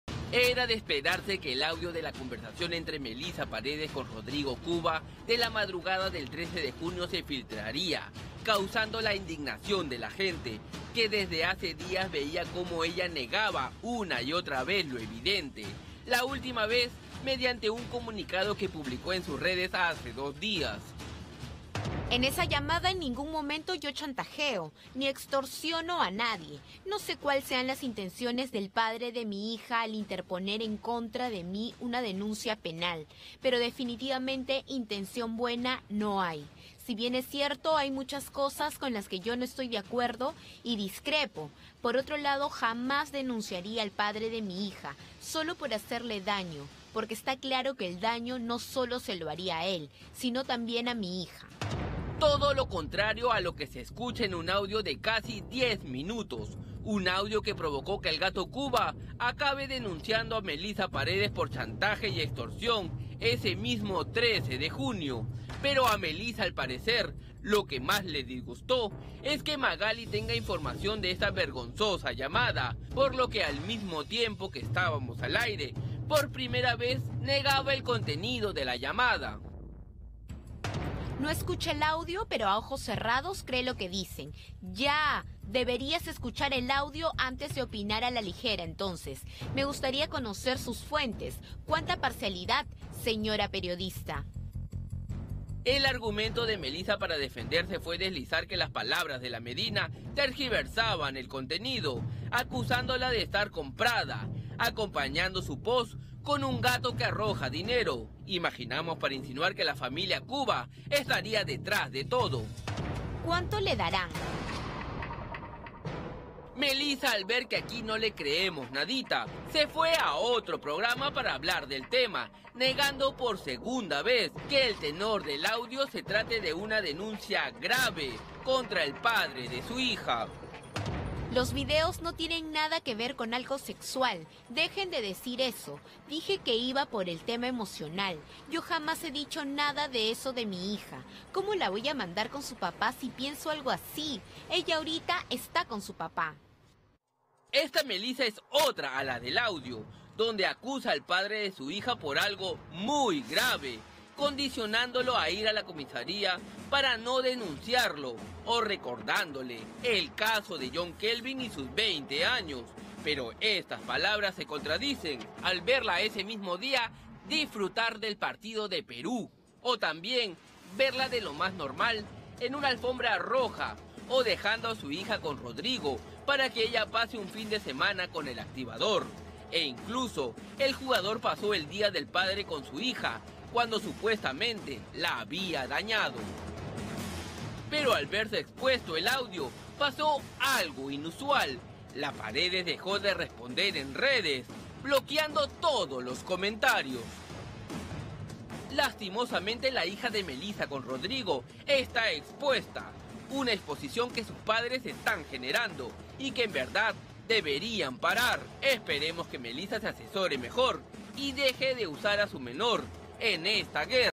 El audio de Melissa Paredes: Las contradicciones de la exesposa de Rodrigo Cuba
Fue filtrado un audio entre una conversación entre Melissa Paredes y Rodrigo Cuba, causando la indignación de la gente, que desde hace días veía como ella negaba lo evidente.